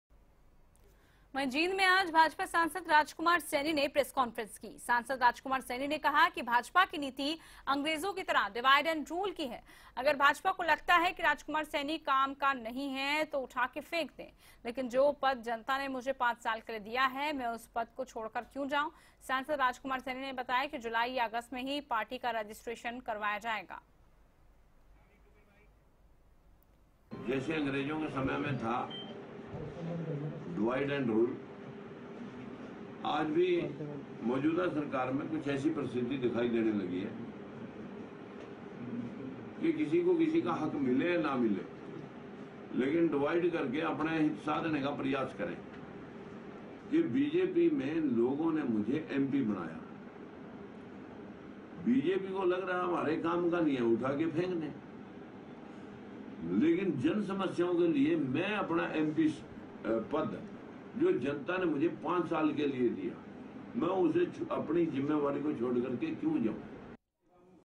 'डिवाइड एंड रूल' है भाजपा की नीति (10/07/2018)
जुलाई या अगस्त में होगा पार्टी का रजिस्ट्रेशन